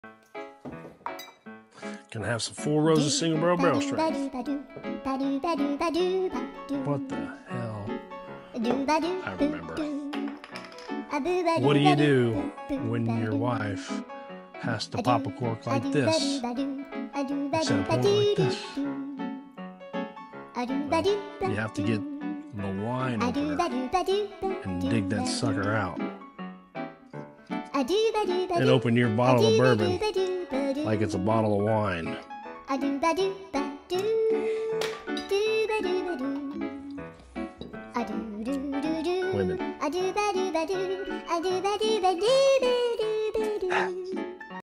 UGH! WOMEN! Amy broke the cork off! How am I going to open it now?
What do ya do when your wife breaks the cork on your bourbon whiskey?